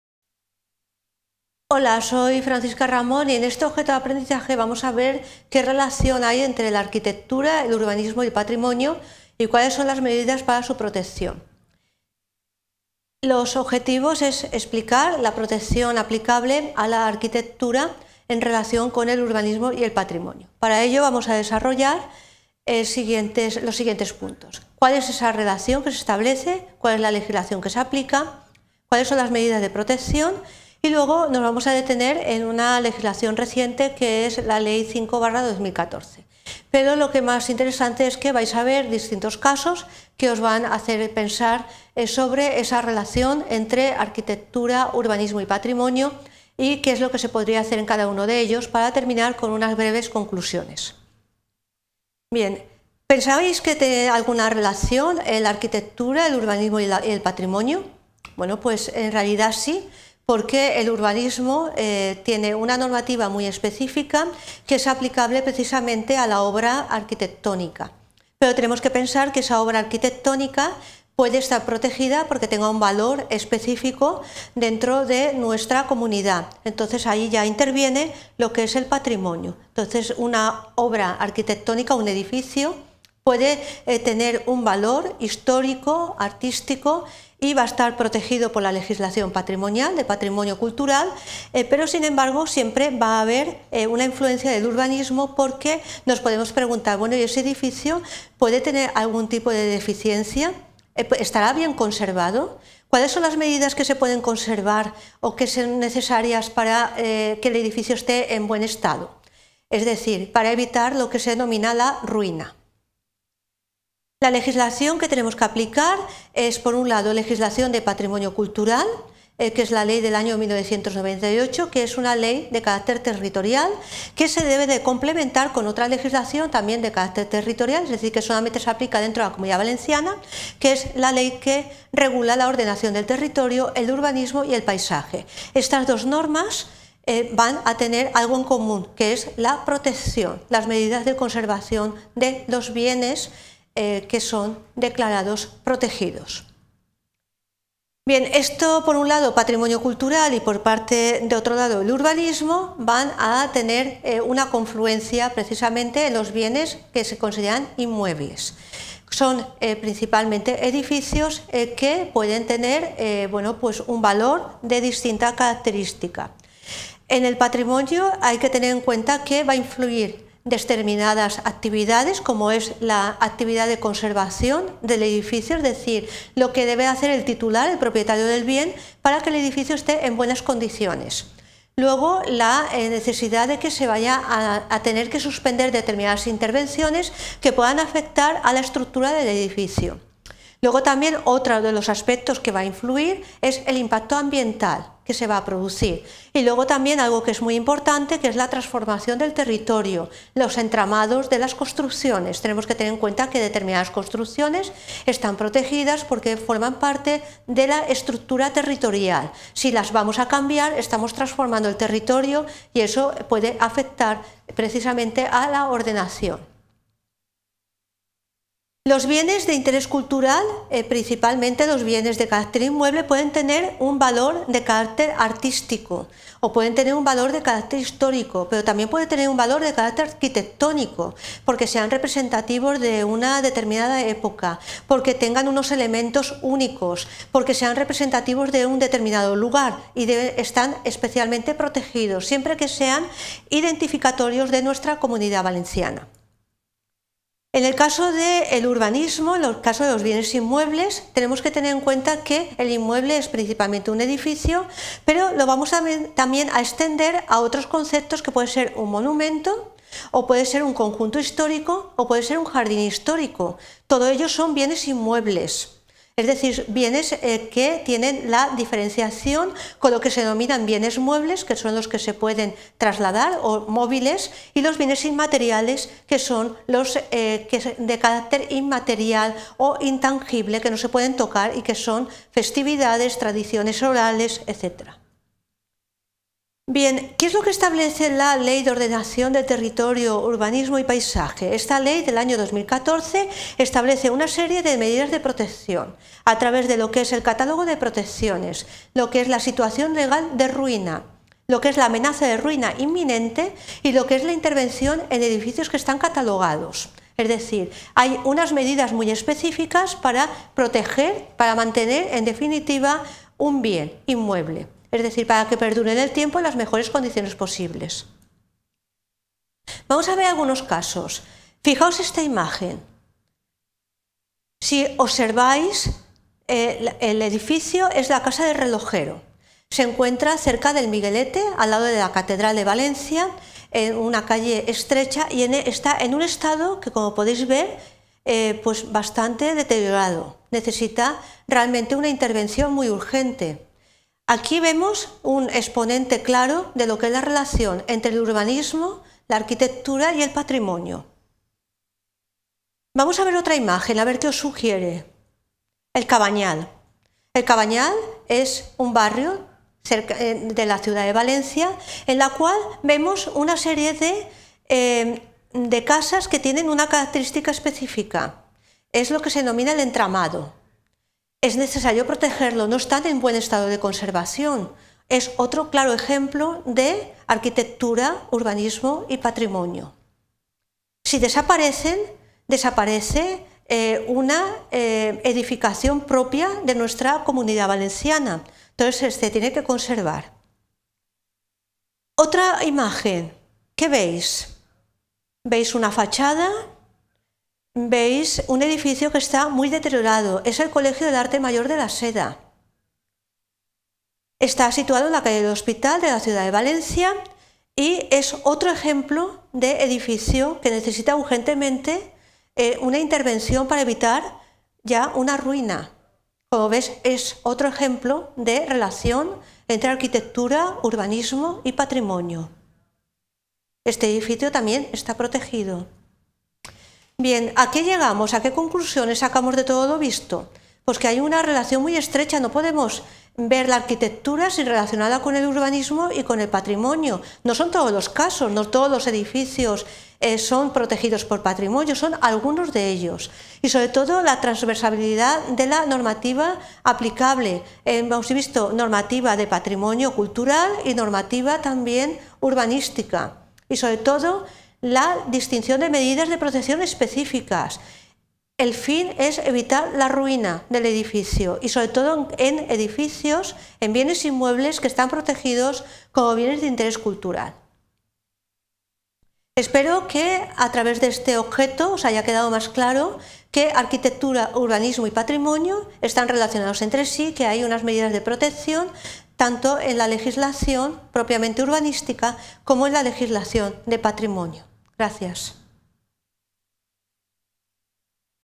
Arquitectura, urbanismo y patrimonio: medidas para su protección| UPV
Título: Arquitectura, urbanismo y patrimonio: medidas para su protección

Descripción: Ramón Fernández, Francisca;

Explicación de la relación entre la arquitectura, el urbanismo y el patrimonio en relación con los inmuebles protegidos y la declaración de ruina

Descripción automática: En este video, la profesora explica la relación entre la arquitectura, el urbanismo y el patrimonio, incluyendo la legislación aplicable y las medidas de protección. Se establece que el urbanismo, que cuenta con una normativa específica, puede afectar a edificios con valor histórico, artístico o arquitectónico que son protegidos por la legislación patrimonial. La ley del Patrimonio Cultural de 1998 y la Ley de Urbanismo y Paisaje de la Comunidad son fundamentales en este contexto, ya que ambas tienen como objetivo la conservación de bienes protegidos.

Se aborda cómo factores como la conservación y el impacto ambiental inciden en la arquitectura del patrimonio. Además, se destaca la importancia de evitar la ruina y cómo ciertas construcciones son esenciales en la estructura territorial. La ley proporciona medidas concretas para el mantenimiento y protección de edificios catalogados. A través de ejemplos de edificaciones, como la Casa del Relojero, el barrio y el Colegio del Arte Mayor, se ilustra la urgencia de intervenciones de conservación.

En conclusión, el video explica que hay una estrecha relación entre la arquitectura, el urbanismo y el patrimonio, no todos los edificios están protegidos, y la normativa que regula la conservación es transversal, buscando evitar la ruina y preservar los bienes de interés cultural.

Autor/a: Ramón Fernández Francisca